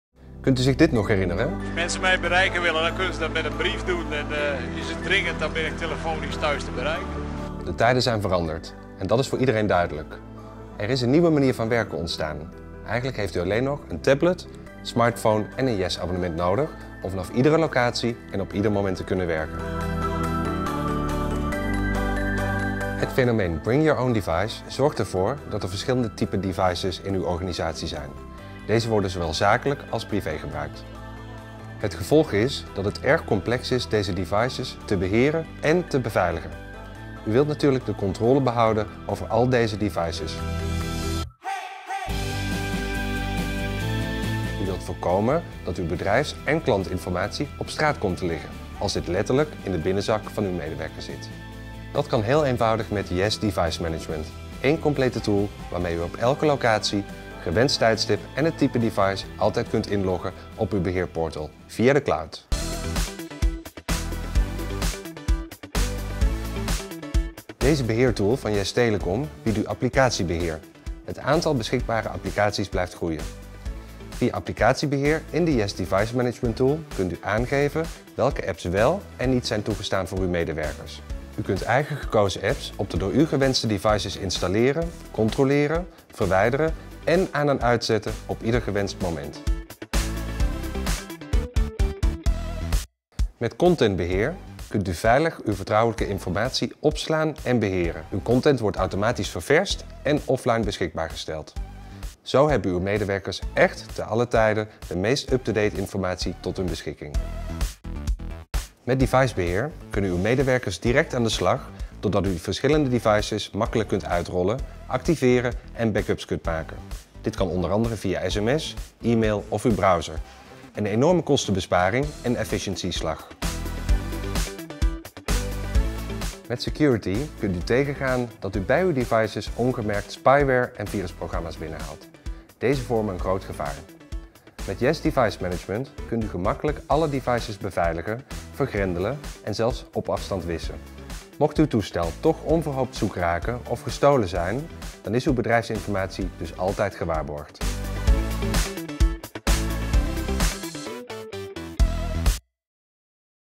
Yes Telecom Device Management 2014
Creative Director: Quierine van Dijk
Director of Photography: Pierre Visser

Client: YES Telecom

A video about YES Telecom's innovative program for effective communication within your company.